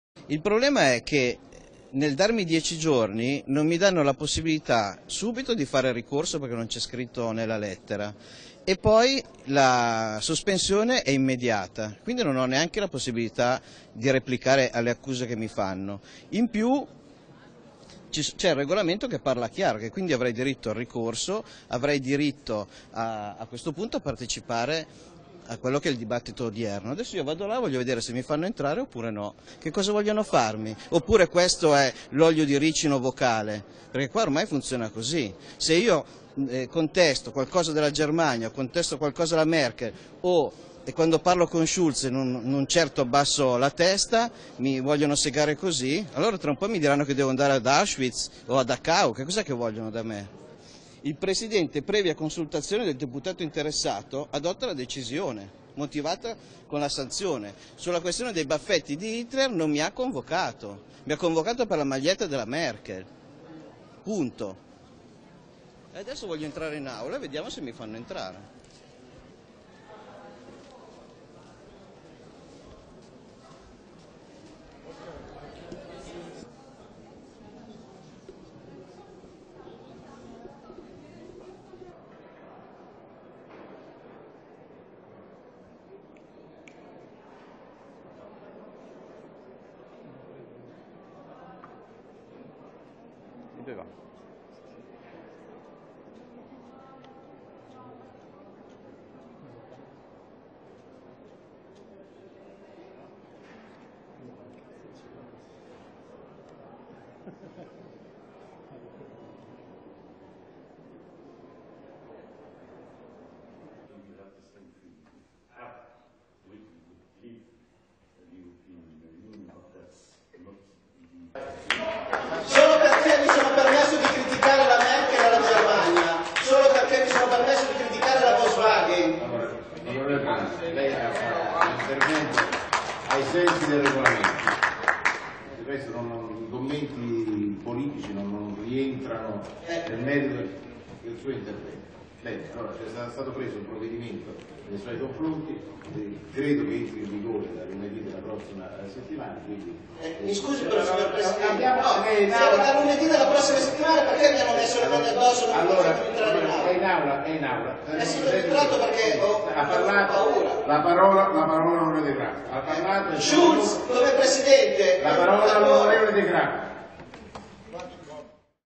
Buonanno in aula al PE - 14 ottobre 2015
Non volevano farmi entrare in aula ma io sono entrato lo stesso!!! Guardate…